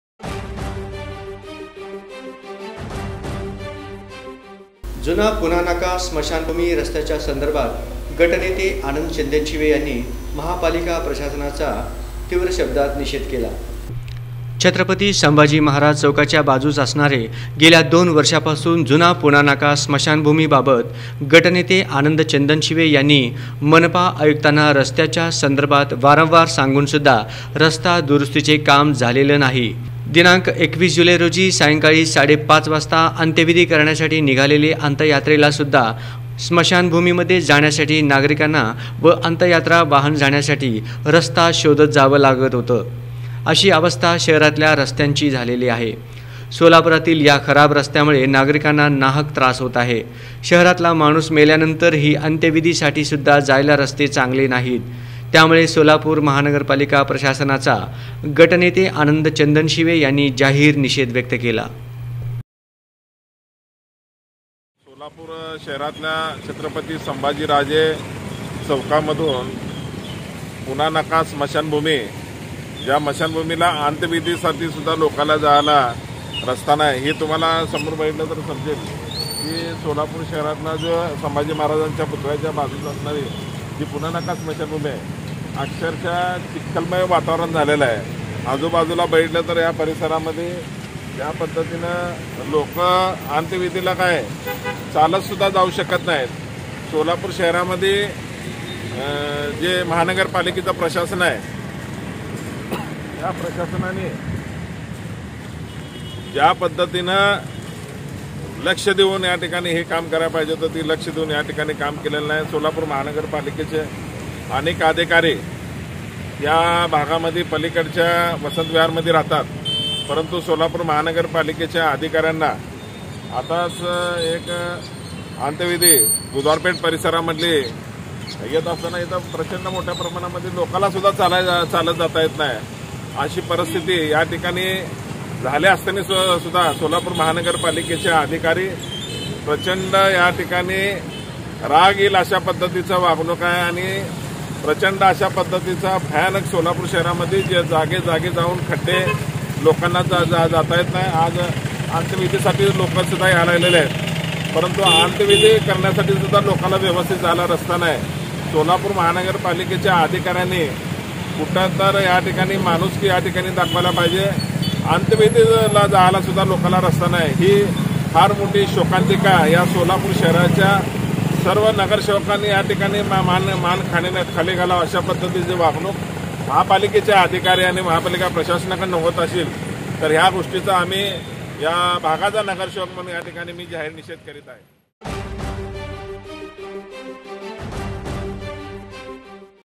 Marathi - माणूस मेल्यानंतरही अंत्यविधीसाठी रास्ता शोधत जावं लागतं; पहा प्रशासनाचा ढिम्म कारभार...
माणूस मेल्यानंतरही अंत्यविधीसाठी रास्ता शोधत जावं लागतं; पहा प्रशासनाचा ढिम्म कारभार...